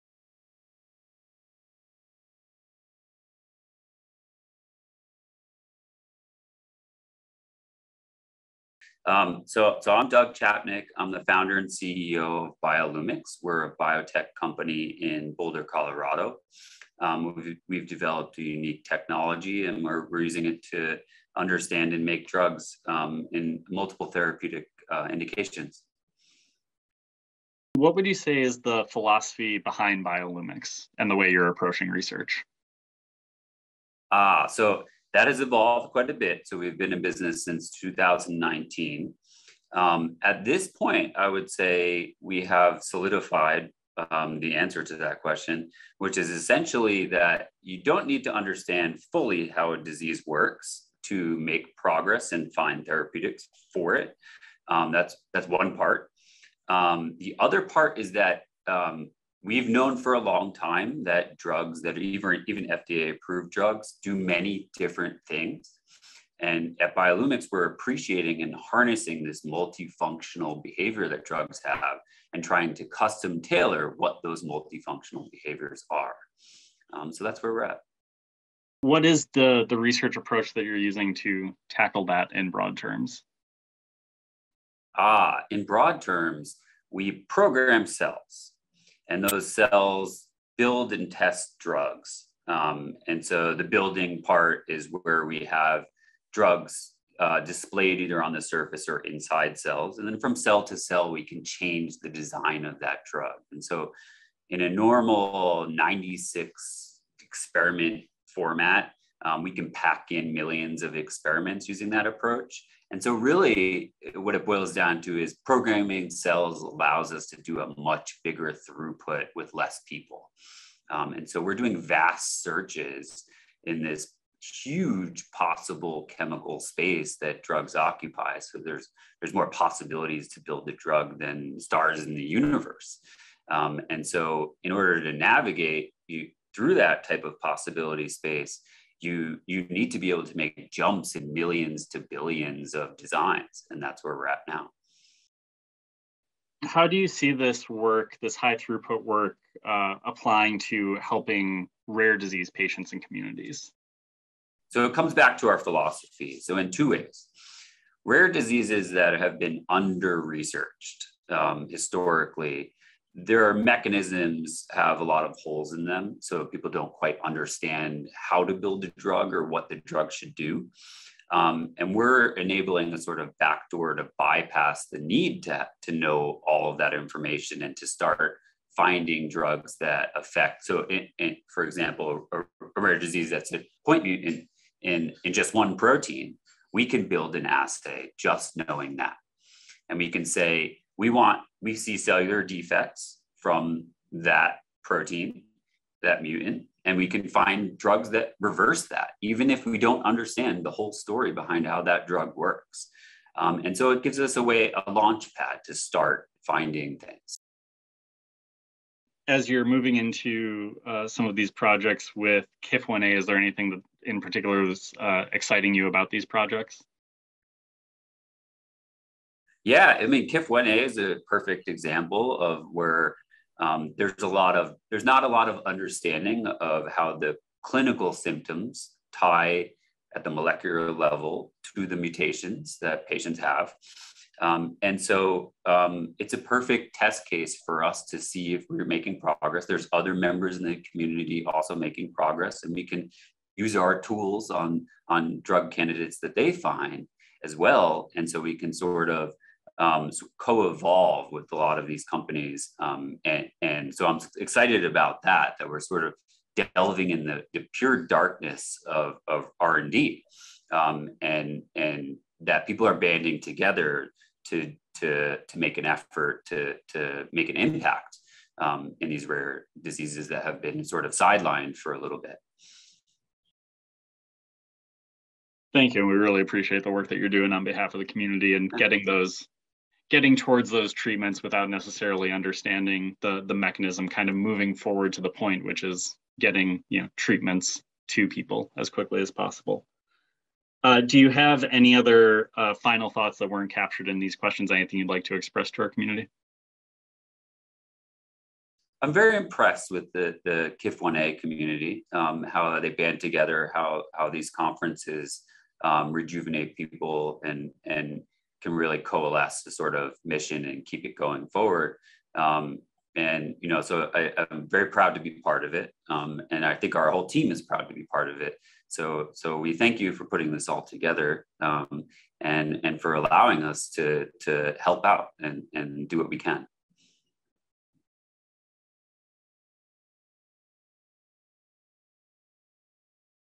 KIF1A.ORG Interview with Dr. Doug Chapnick
Doug Chapnick, PhD, CEO of BioLoomics, discusses how high throughput screening can help rare disease communities.